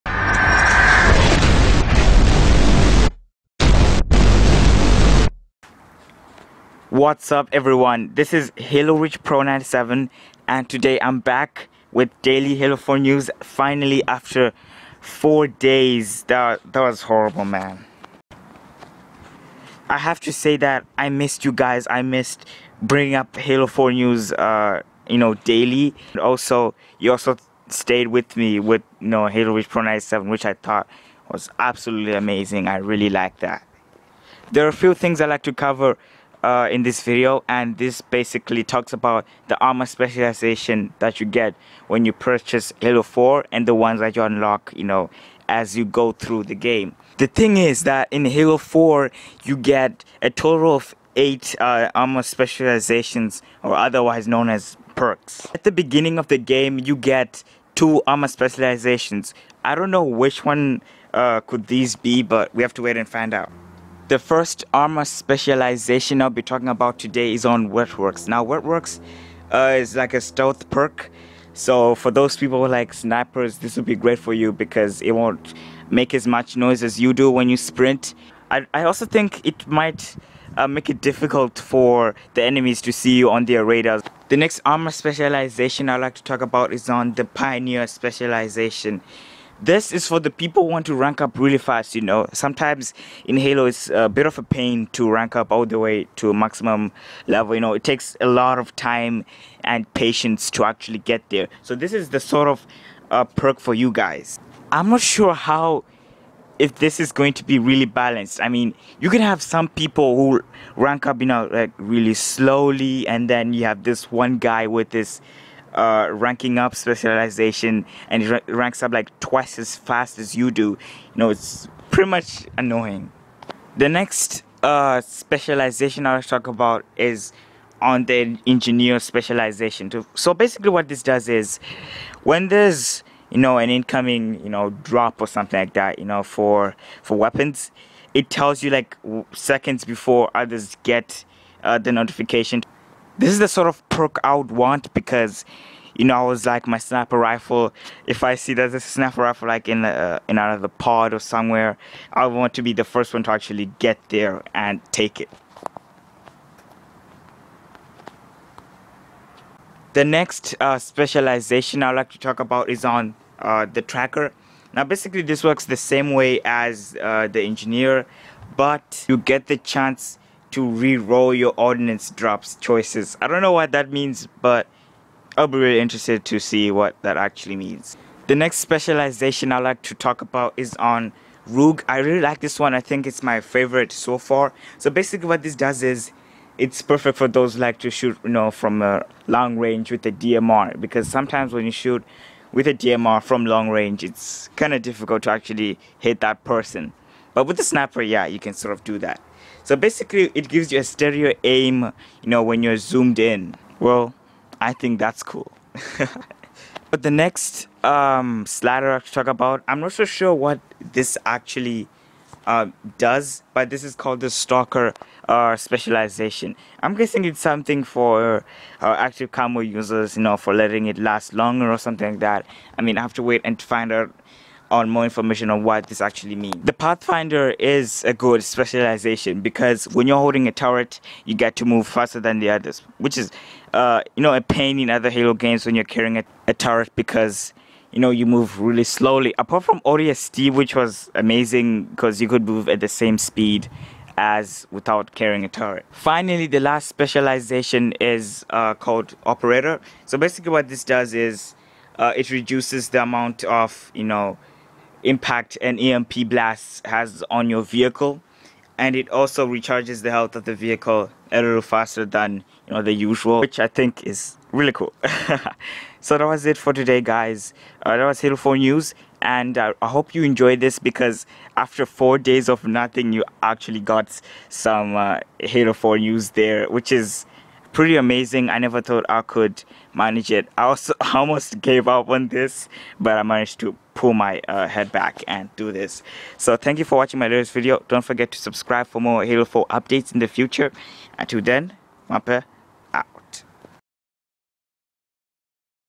Halo 4 News: New Specializations or "Perks" confirmed + Details and Explained
Today's Episode of daily Halo 4 News covers the new confirmed Specializations in Halo 4 in detail such as the Engineer specialization that was confirmed.